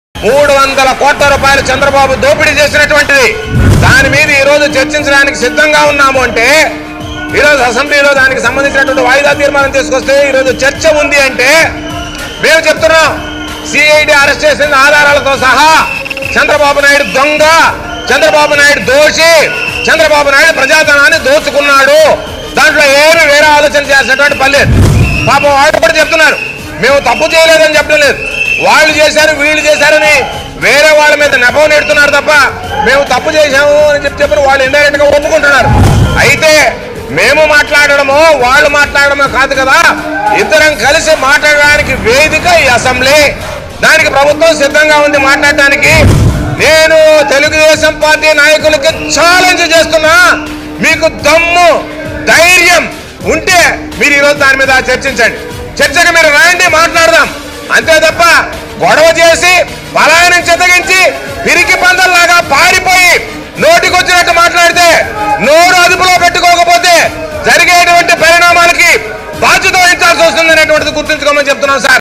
టీడీపీకి దమ్ముంటే చర్చకు రావాలి // టీడీపీ ఎమ్మెల్యేలు సభలో నానాయాగీ చేస్తున్నారు
Watch : టీడీపీకి దమ్ముంటే చర్చకు రావాలి // టీడీపీ ఎమ్మెల్యేలు సభలో నానాయాగీ చేస్తున్నారు